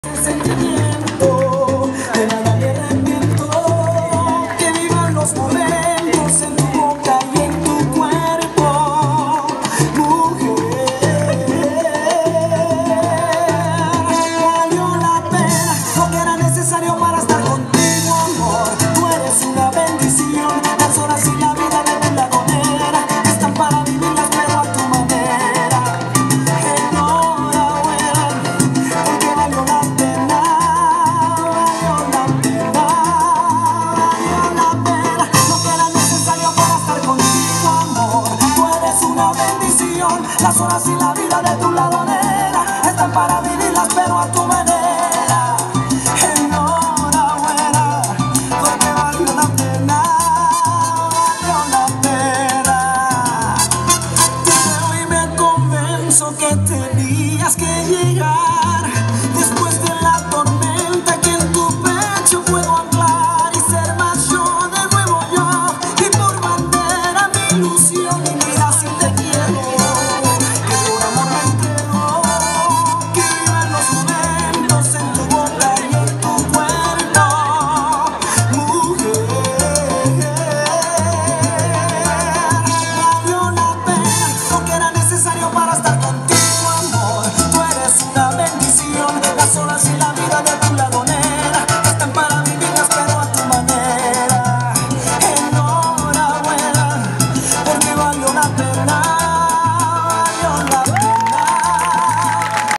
LVN 2015: Amani Mehrez's Salsa Dance at Miss & Mr LVN 2015
Video: Asma Ben Aîcha
Play it in HD for a better quality!